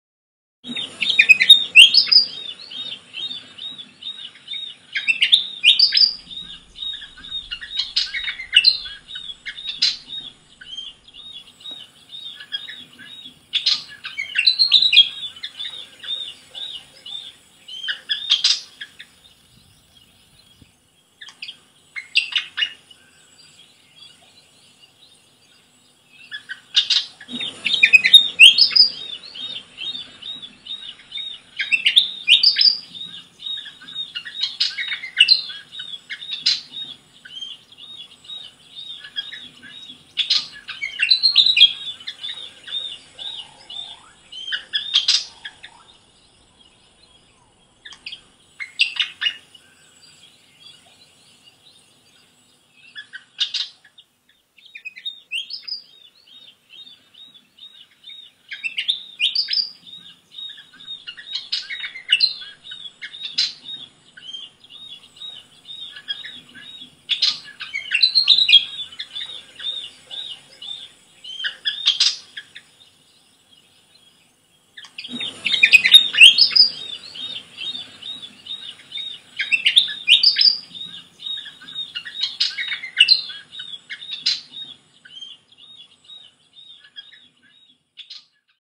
Willie Wag Tail vs Crocodile
Willie Wagtail filmed at Fog Dam Northern Territory, Australia